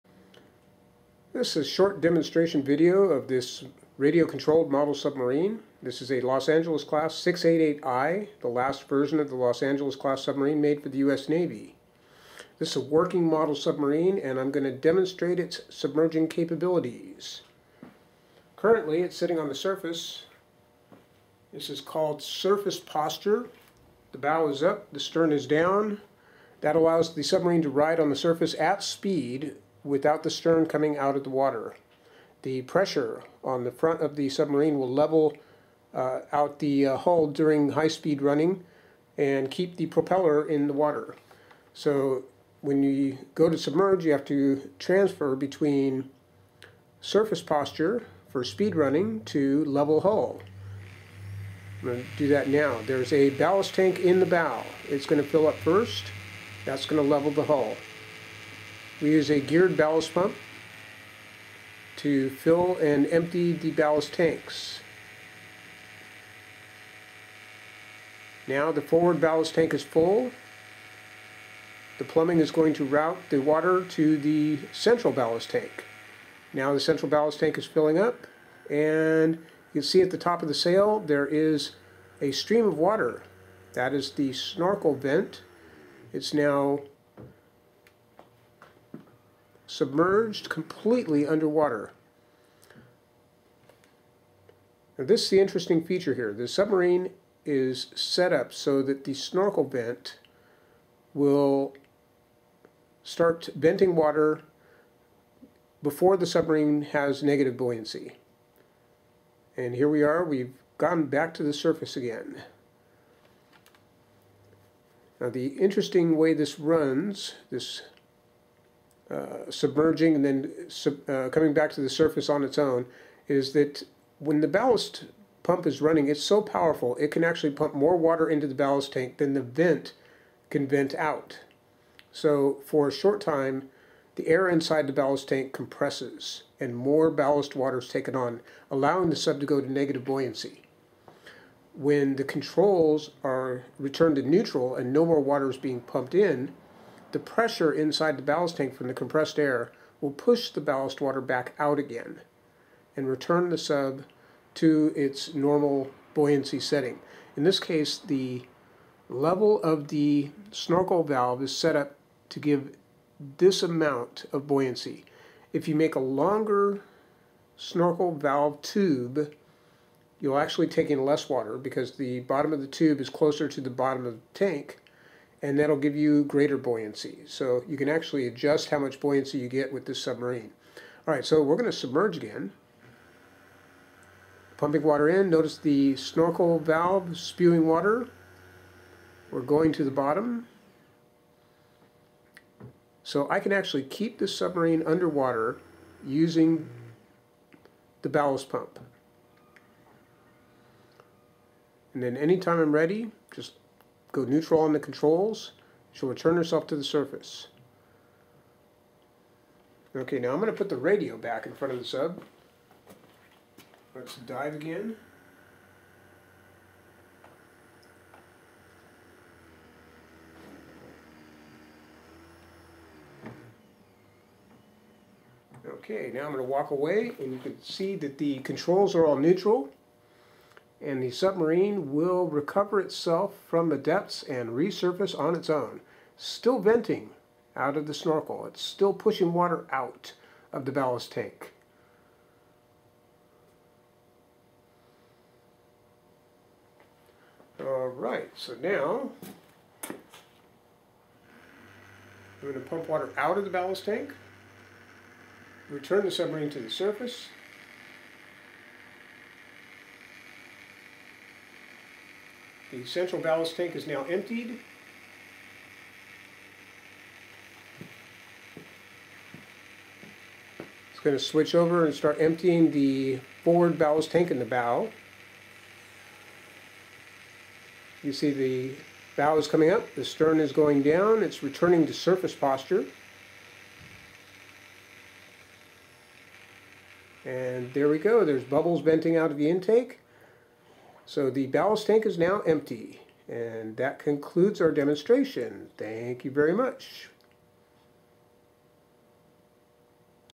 Los Angeles Class 688i fast attack submarine model: Basic Dive Demonstration.
Basic demonstration of the 1/100 scale Los Angeles Class 688i RC Submarine model submerging and surfacing capabilities.

This submarine model is my own design and created from molds I designed and built.